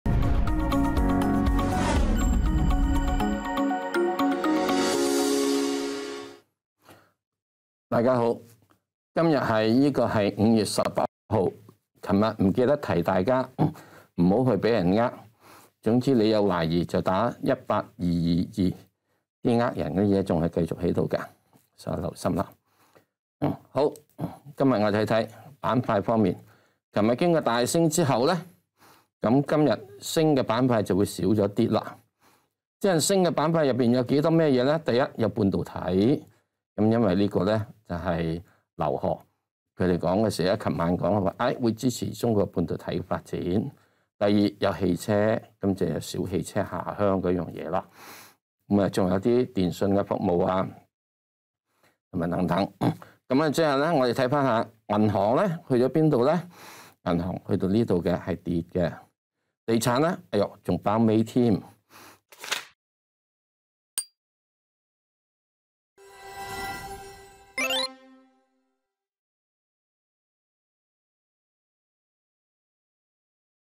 【石Sir午市閒談】今日推介新地、百威亞太、中移動
恒生指數中午收報20471點，跌130點。期指應如何部署？《香港經濟日報》專欄作者石鏡泉（石Sir）推介新地（00016）、百威亞太（01876）、中移動（00941），詳情待石Sir為你分析。